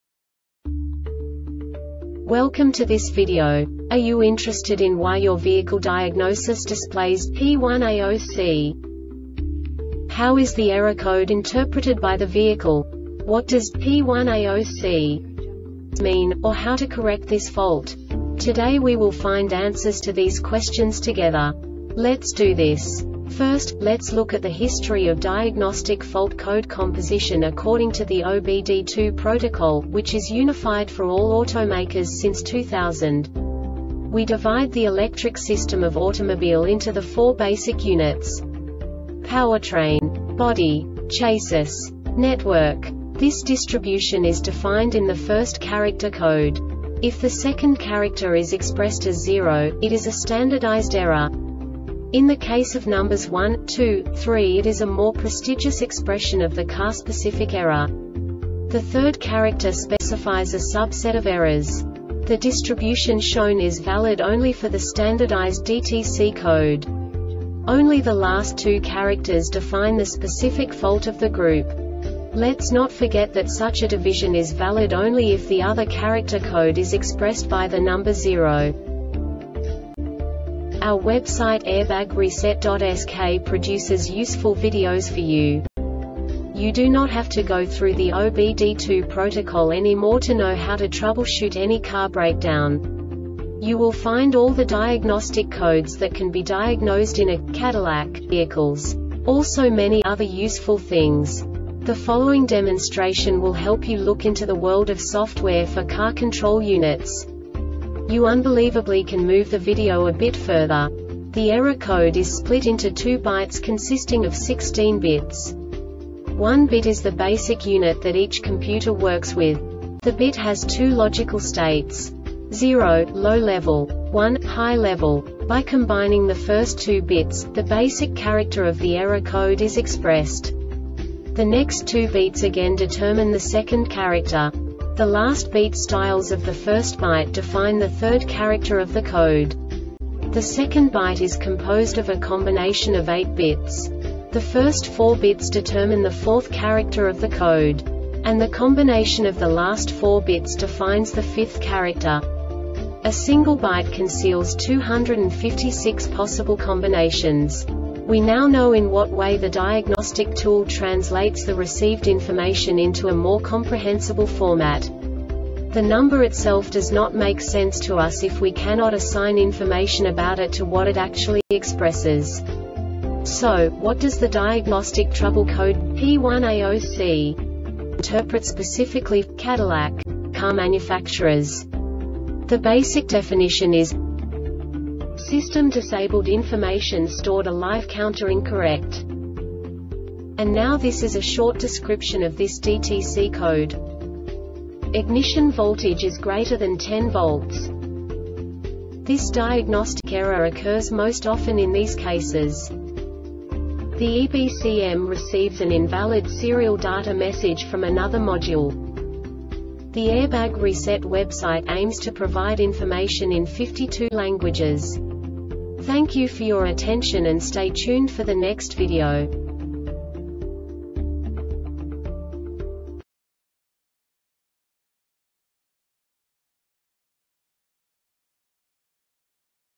DTC cadillac P1A0C Short Explanation
Contents:
0:21 Basic DTC analysis according to OBD2 protocol standard.
1:48 Insight into programming
2:58 A diagnostically understandable description of the Diagnostic Trouble Code
3:05 Basic error definition

Make:
Cadillac
Code:
P1A0C
Definition:
System Disabled Information Stored Alive Counter Incorrect
Description:
Ignition voltage is greater than 10 volts.
Cause:
The EBCM receives an invalid serial data message from another module.